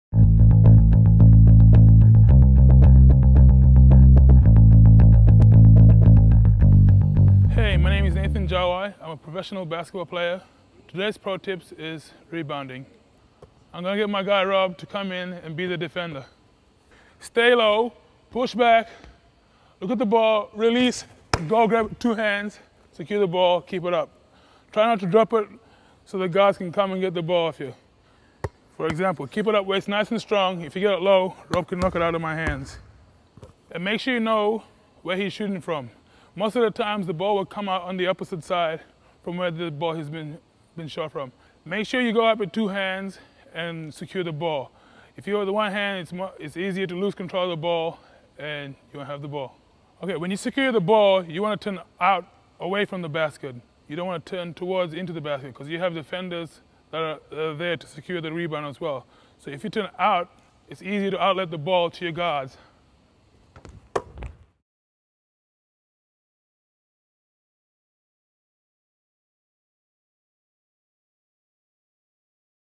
Nathan Jawai - Rebounding
In this Pro Tips 4U sports training sample video, Nathan Jawai, Power Forward/Center for the Serbian team KK Partizan, goes over Rebounding. Learn the secrets of boxing out and the proper form of staying low, pushing back, following the ball, releasing and why you need to go after the ball and grab with two hands. Nathan also shows you which way you should turn after the rebound. Lastly, Nathan demonstrates how smaller players can box out for a rebound.